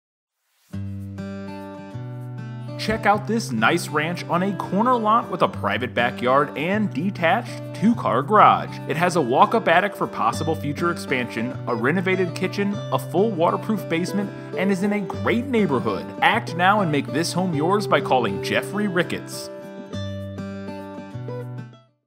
204 Gold Street Shillington, PA | ColdwellBankerHomes.com
204 Gold Street, Shillington, PA

Corner lot, private backyard, detached two-car garage, walk-up attic for possible future expansion, renovated kitchen, full waterproofed basement, great neighborhood!